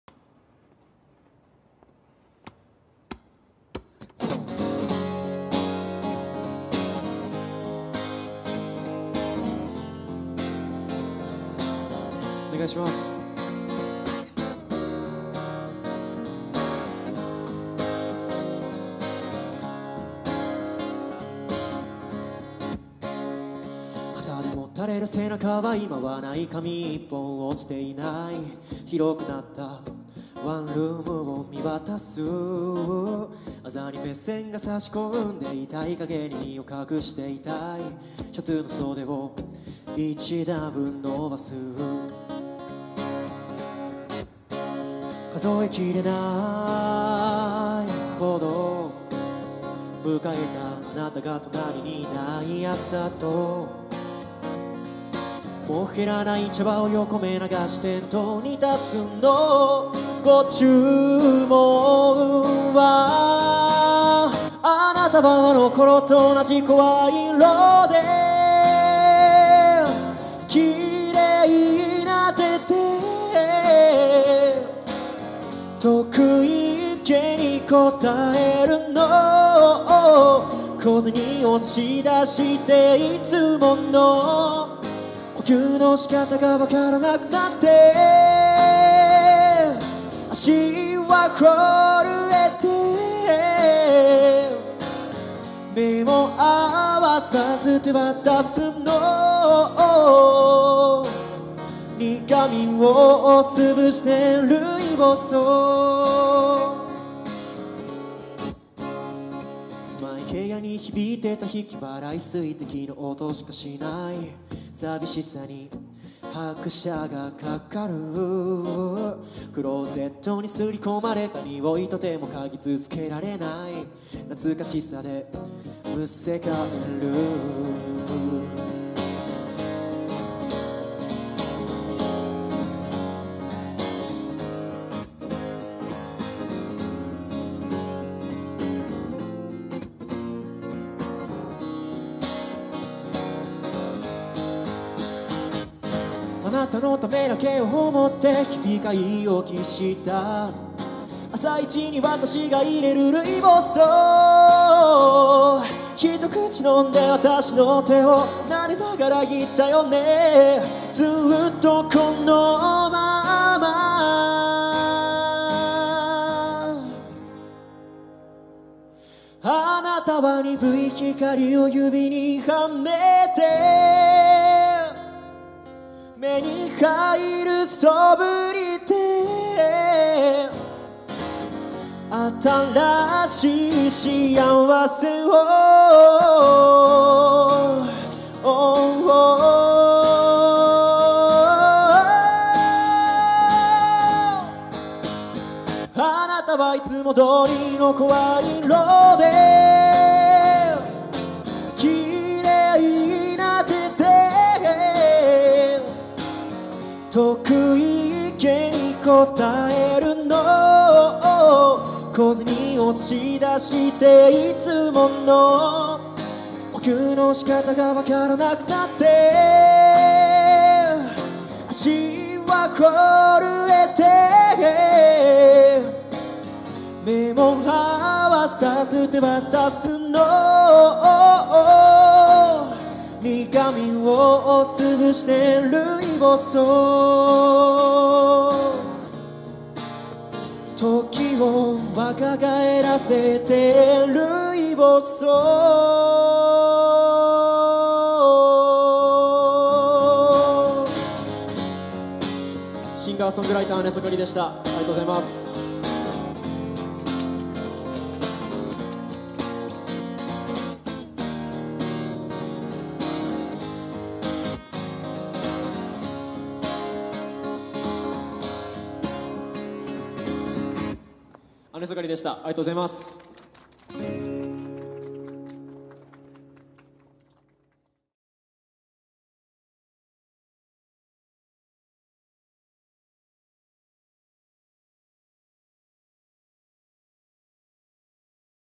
【 歌うま 】テレ東「めをとじ」で話題の歌声！" あねそかり " 路上ライブ 4K映像
【 歌うま 】テレ東「めをとじ」で話題の歌声！" あねそかり " 路上ライブ 4K映像 Busking in Japan

③注目のアーティスト動画

④コラボ企画動画